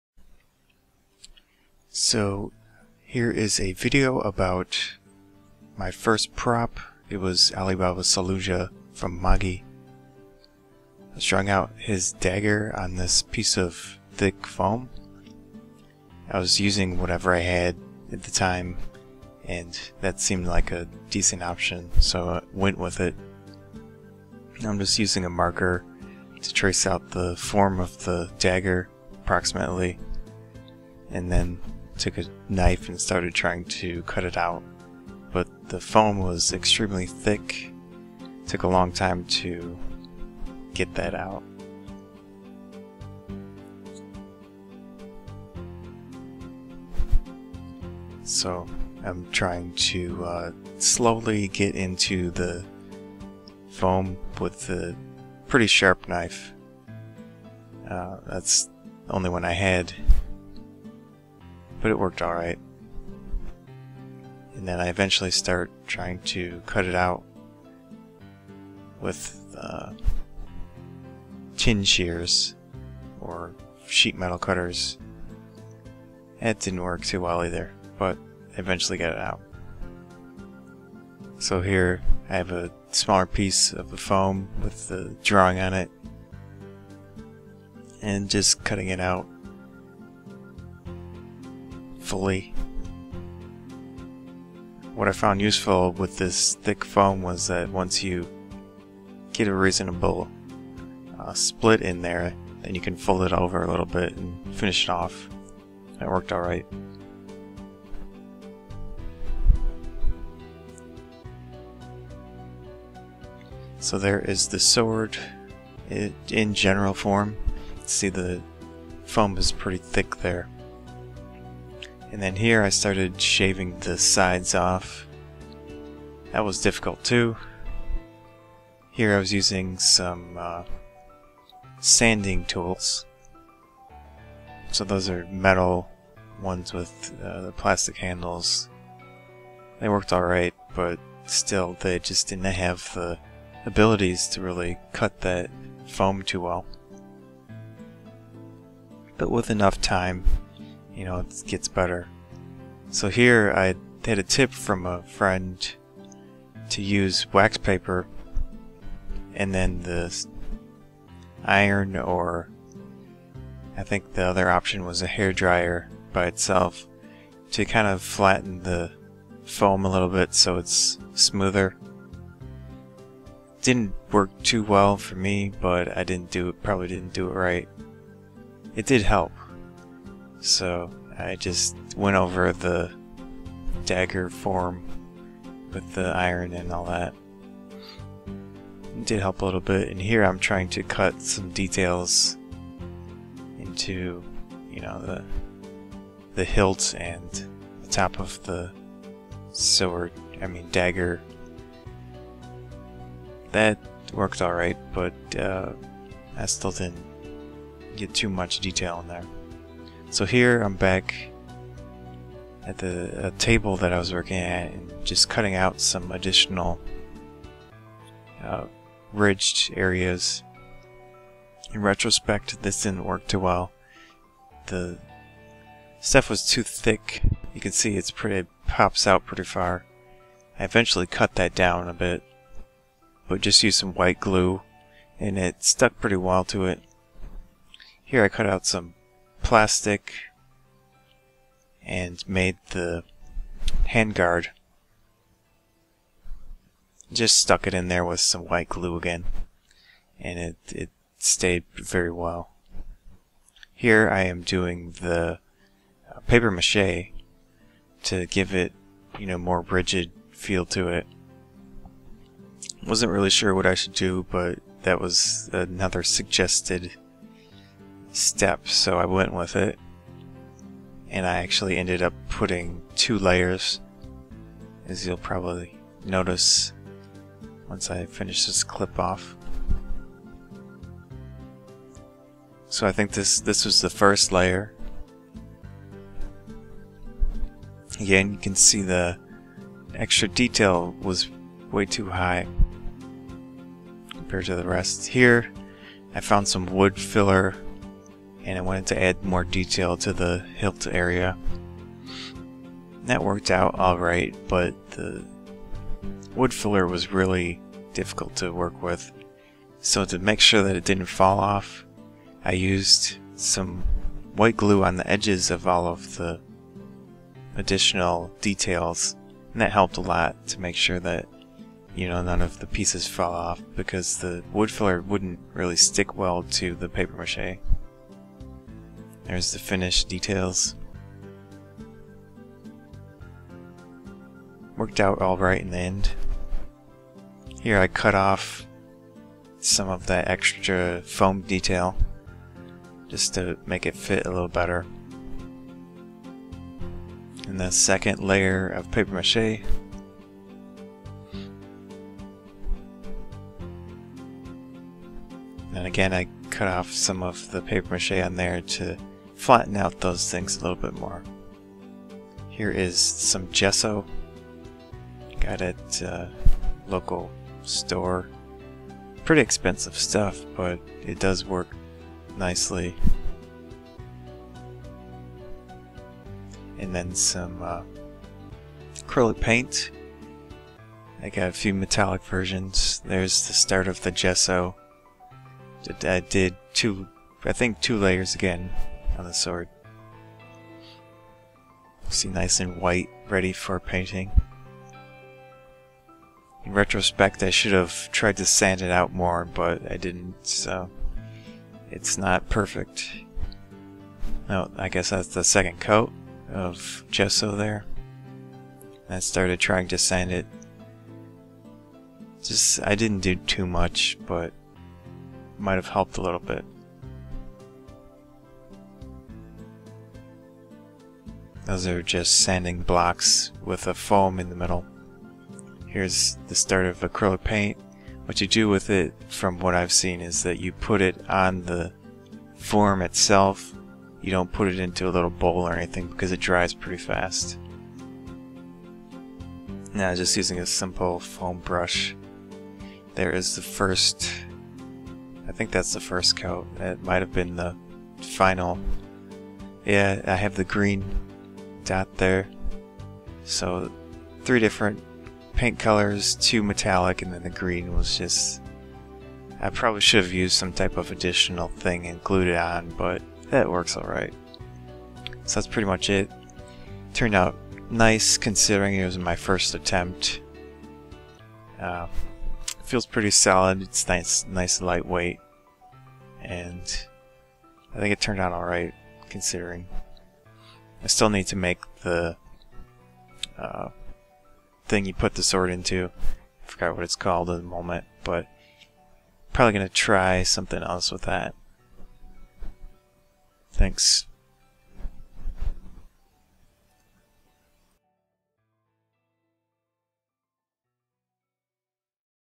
Cosplay Log: Alibaba Dagger Prop
Here is a log of clips I took while making my first prop for an Alibaba Saluja cosplay (Magi anime and manga). It turned out alright, but could have been a bit better if I had sanded it more. I decided to not put the Djinn symbol on the dagger. Also, I forgot to mention in the video that I did put two coats of acrylic based sealant (from Hobby Lobby, Martha Stewart branded clear coat).